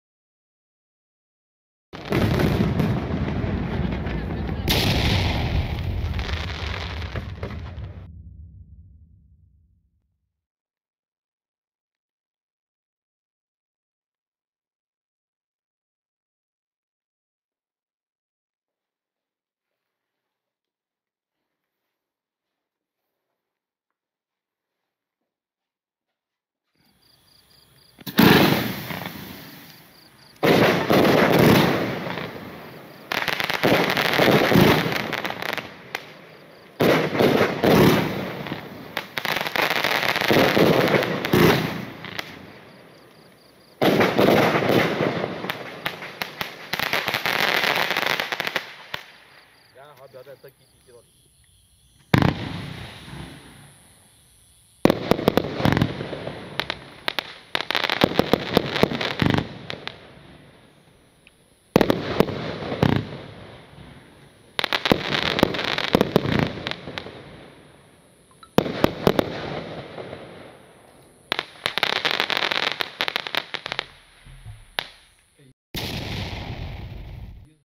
Funke- Chrys Rain F-1
Geniale Fächerbatterie aus dem Hause Funke mit unterschiedlichen Feuertöpfen zu Goldschweifaufstieg und Verwandlung zu Päonien oder Blinkern jeweils mit Goldregen mit Silbercracker-Spitzen